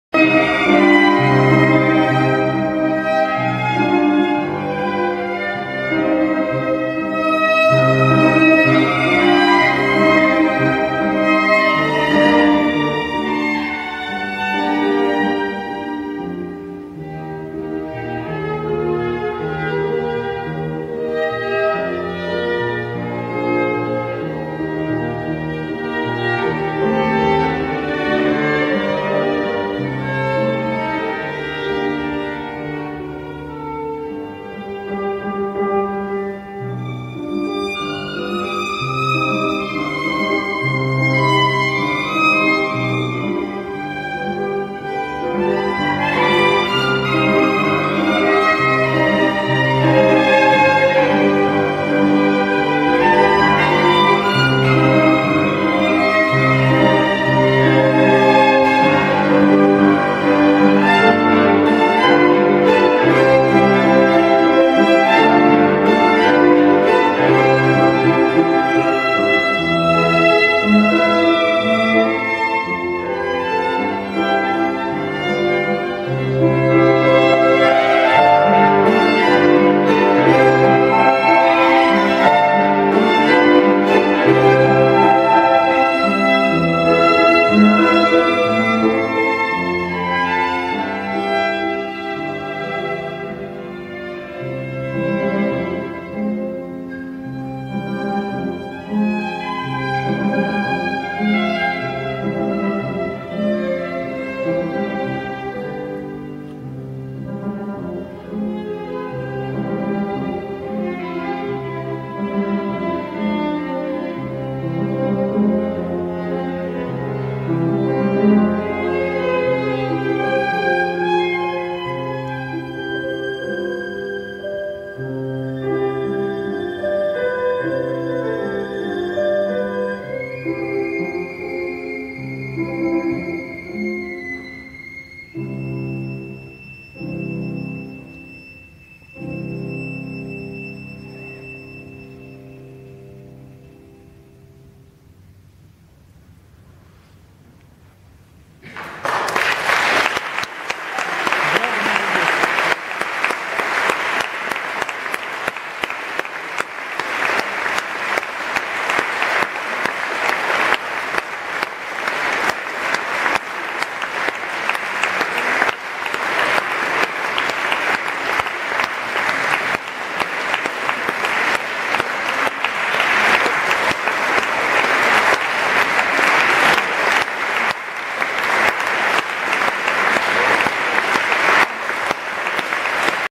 Ana Maria Valderrama and Peter Laul
Ana Maria Valderrama and Peter Laul in the Small Hall of St.Petersburg Philharmonia (Russia) on 30 September 2012.